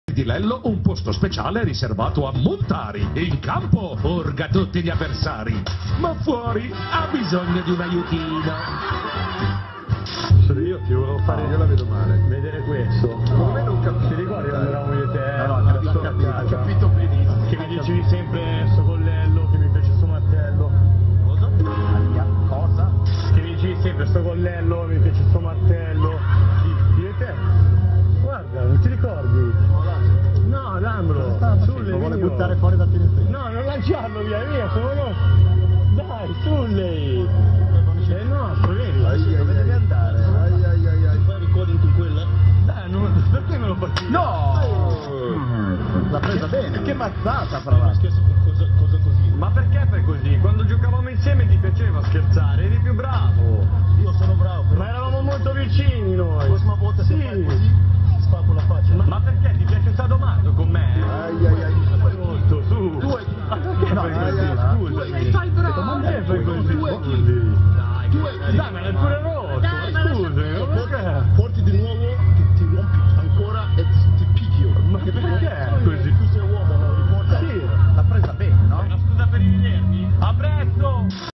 Sulley Muntari destroys pranker's ipad
Muntari in another fight?? Naa he just lost his temper over some silly prank... lol
for latest music downloads